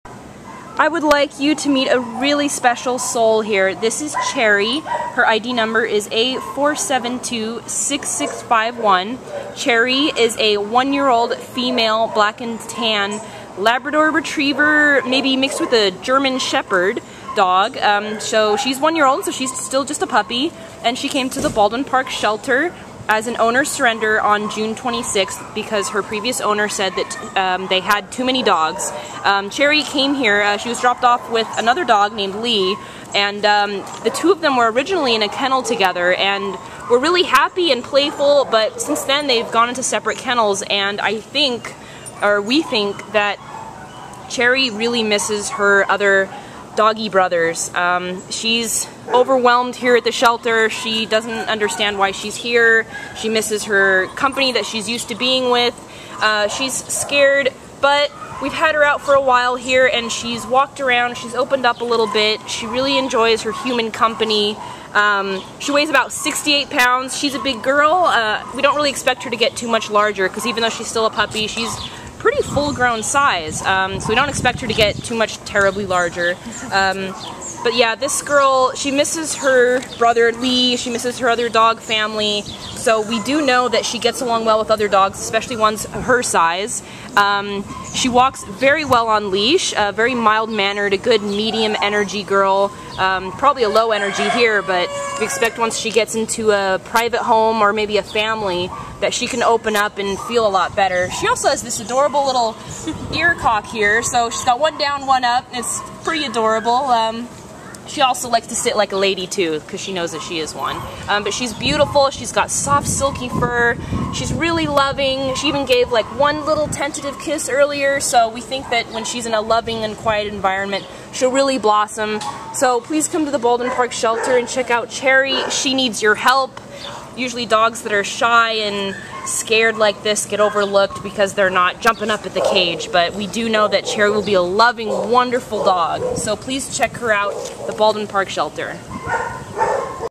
A4726651 Cherry
A4726651 Cherry is a serene and soulful 1-year-old black and tan female Labrador Retriever/ German Shepherd mix puppy who was left at the Baldwin Park Animal Care Center with Lee [A4726650] on June 26th because her former owner had too many dogs. Weighing 68 pounds, Cherry is scared from the shelter environment, but it is easy to see her gentle and sweet nature. She walks very well on leash and since she came from a home with multiple dogs [many we know were of a similar size], we know she loves canine companionship.  The best of both breeds, this snuggly girl is a prime choice for teaching tricks and taking hikes. She is bound to become more lively once she is in a loving home. Cherry will make a perfect indoor companion for a family or couple living in a private home.